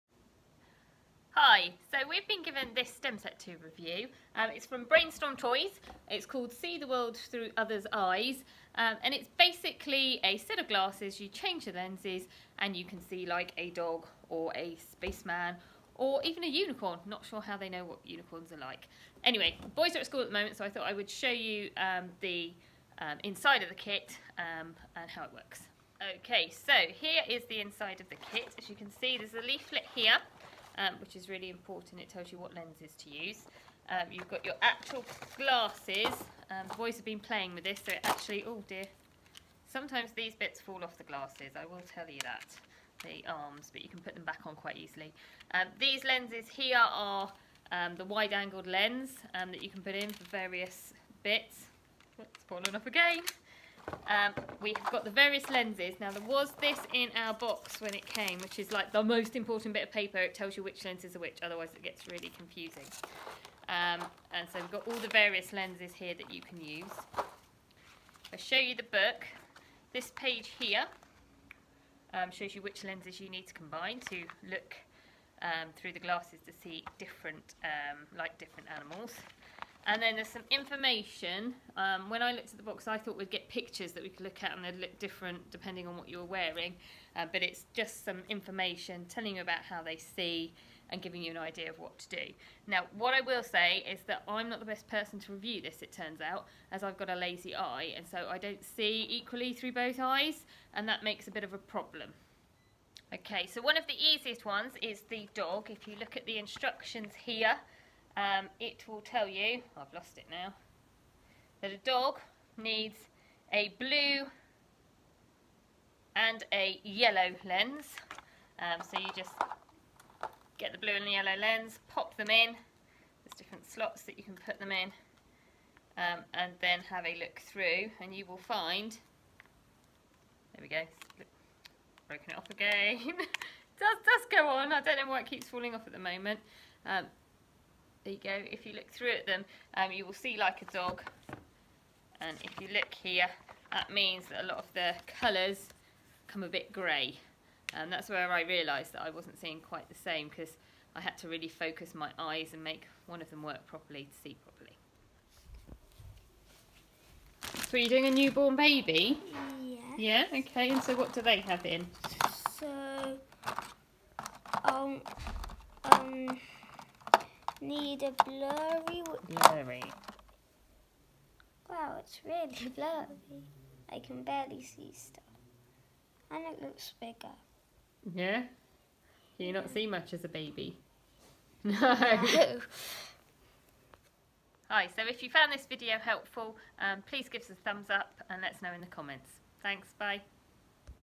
See Through Others' Eyes Review
A look in the box of the Brainstorm STEM set see through others' eyes. We were sent the product free of charge in order to do a review